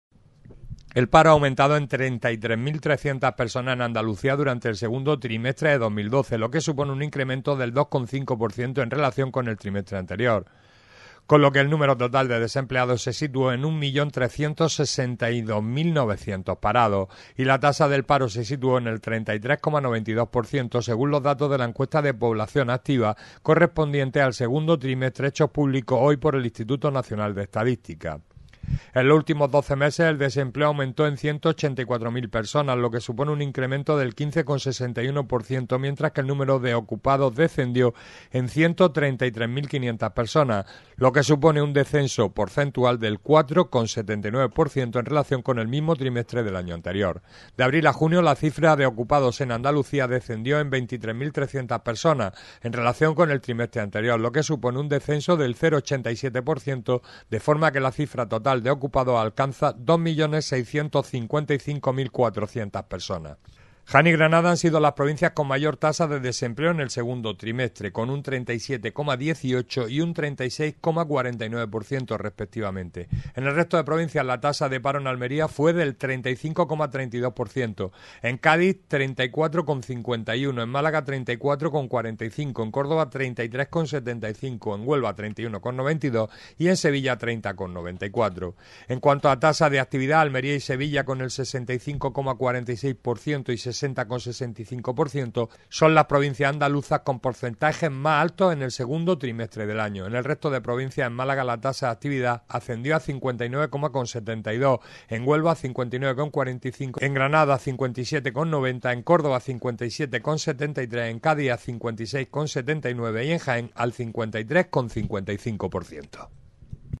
Almería Noticias Canal 28 TV -Almería sobrepasa el 35 por ciento en cifras de paro según la EPA
Para más información visite junto a Córdoba ostenta las tasa de paro más altas de nuestra comunidad autónoma